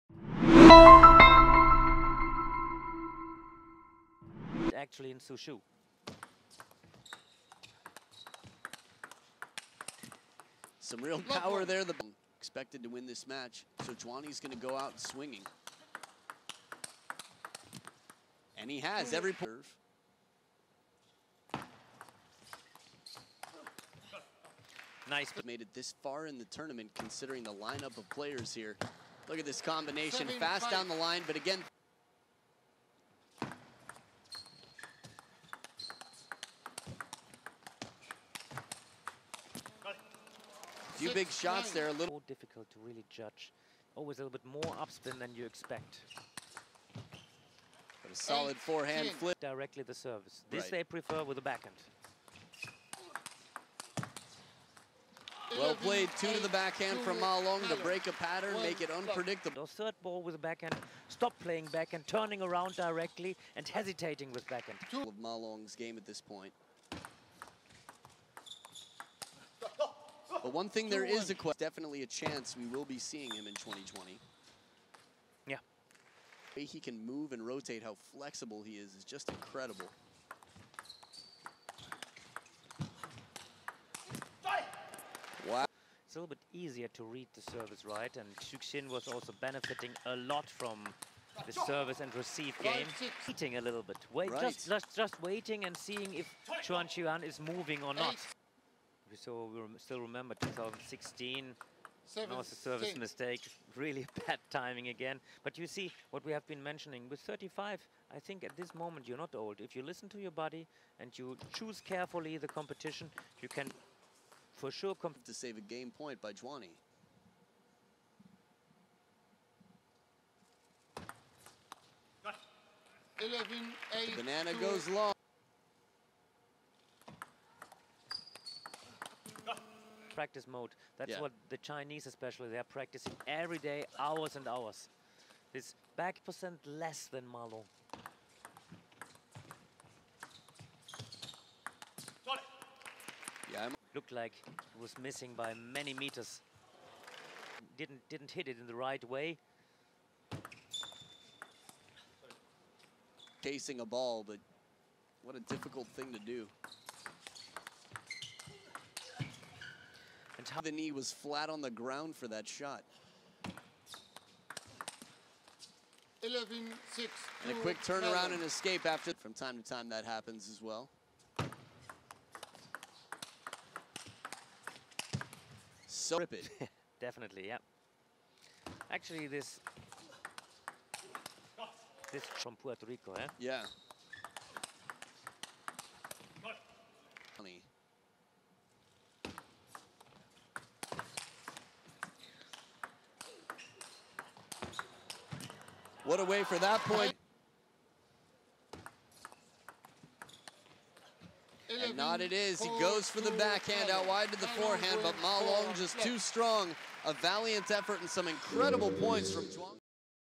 2017 Qatar Open Highlights: Ma Long vs Chuang Chih-Yuan (1/2)
Review all the highlights from the Ma Long vs Chuang Chih-Yuan (1/2) from the 2017 Qatar Open